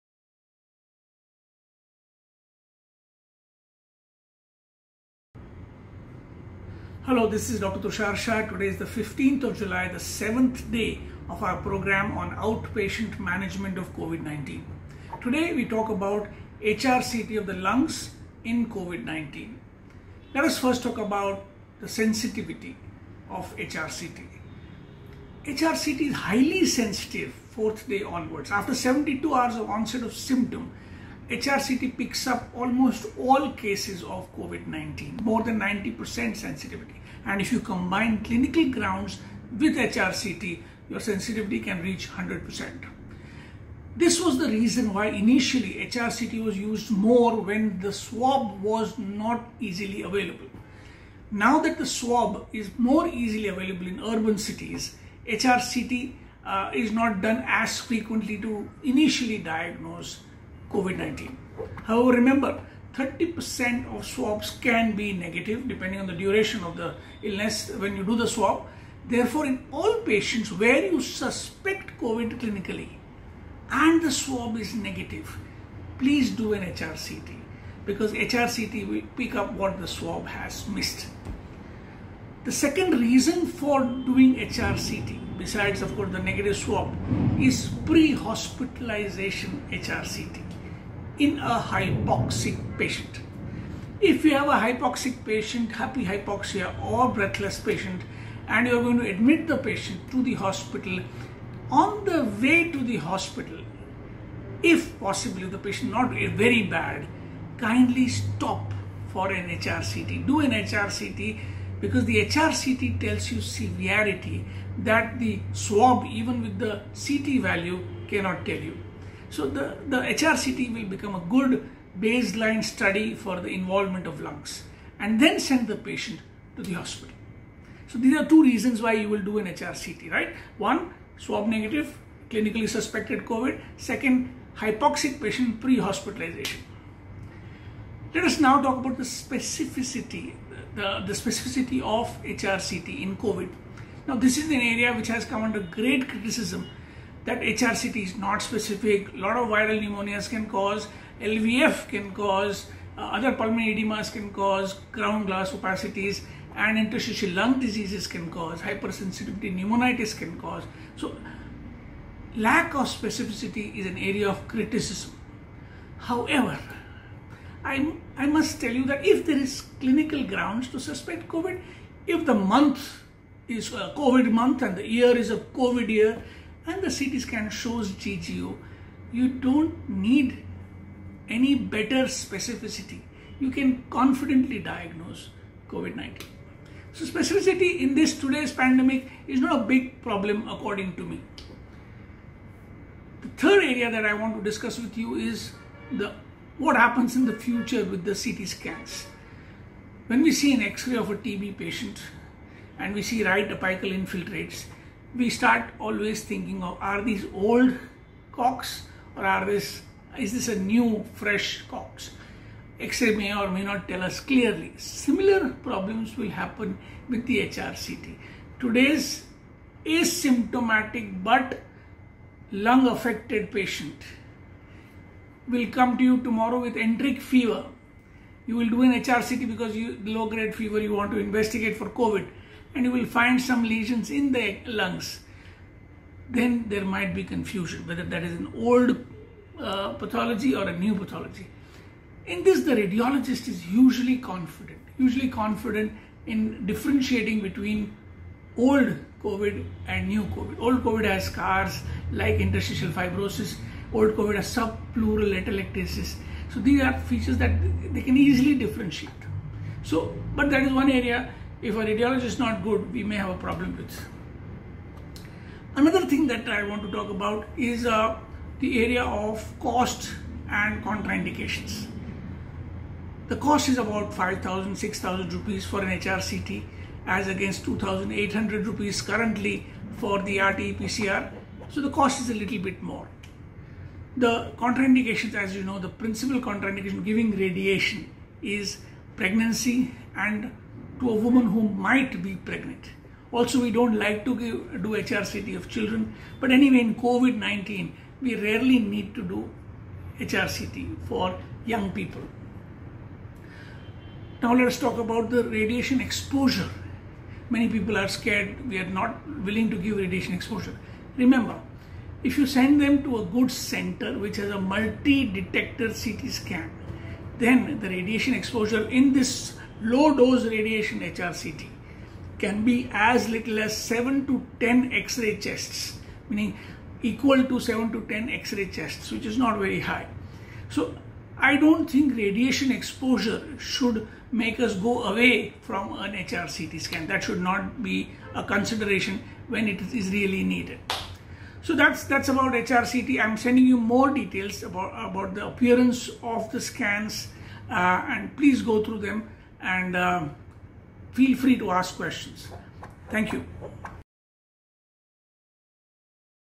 Part 13 - Investigations in COVID19 - HRCT 1/2
Part of the free learning programme on OUTPATIENT DIAGNOSIS AND MANAGEMENT OF COVID-19.
Conducted by dr tushar shah, MD Physician via THE STUDY GROUP BY PiE on the Telegram app.

The intention was  to increase the population of "armchair physicians" capable of correctly diagnosing Covid-19 and then providing  home management in patients who don't need hospitalisation.